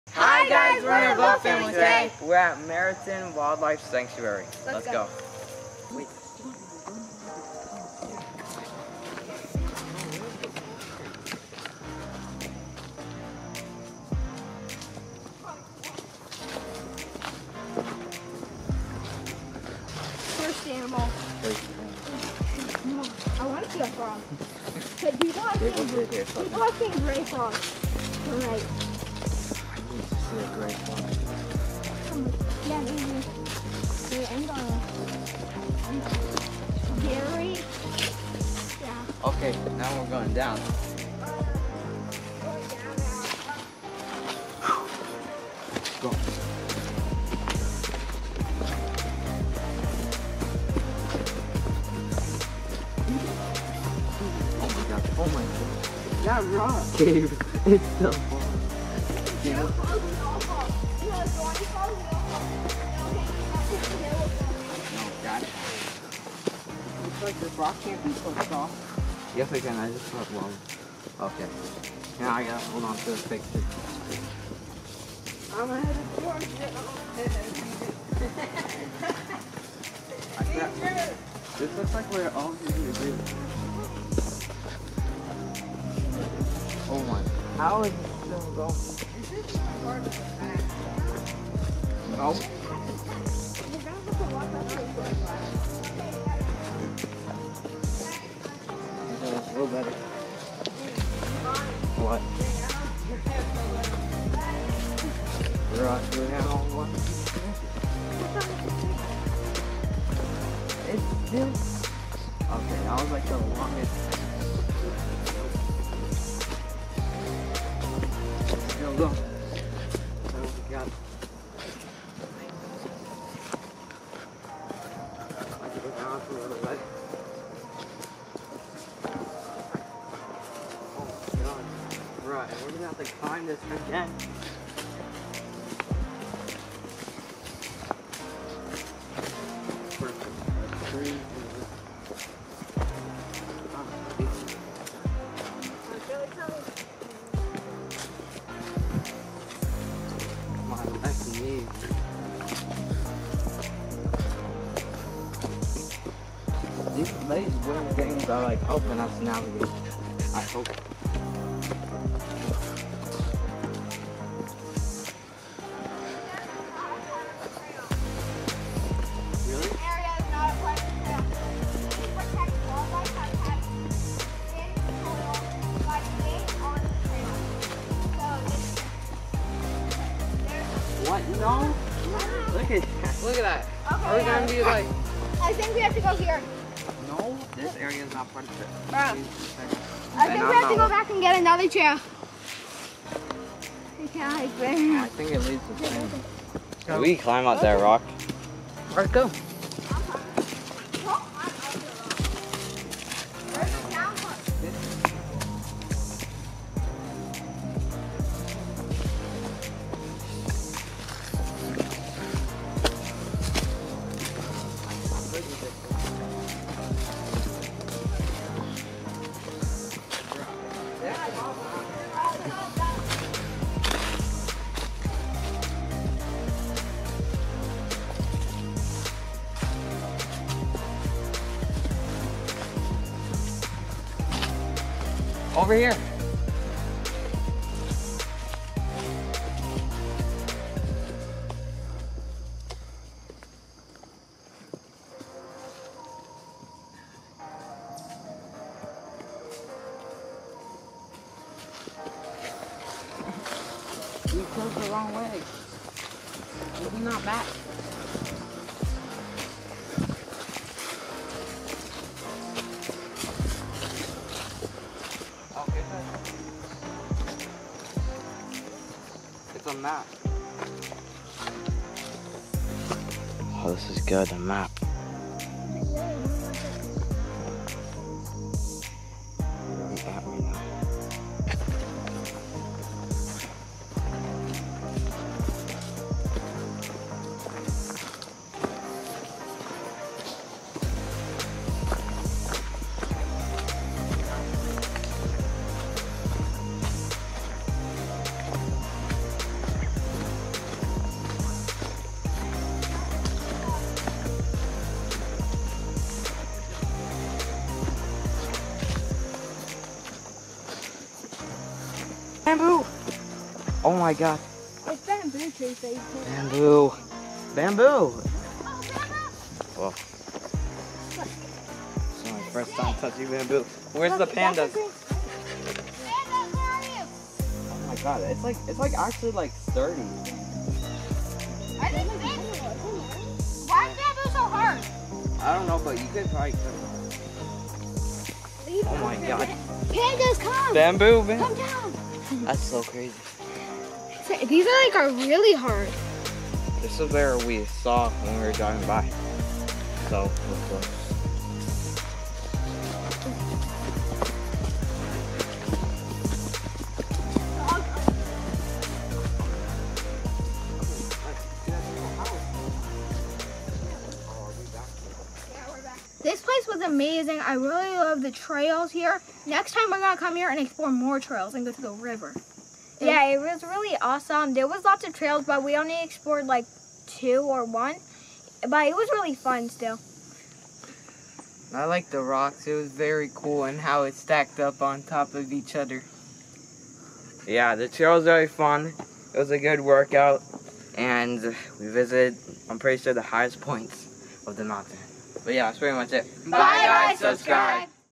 Mariton Wildlife Sanctuary
Natural Lands Mariton Wildlife Sanctuary
Mariton is perched on the steep slopes of Bougher Hill overlooking the scenic Delaware River. 200 acres. A highlight of the preserve’s trail system is the River Lookout Trail, with a seasonal overlook of both the Delaware Canal and the Delaware River 300 feet below.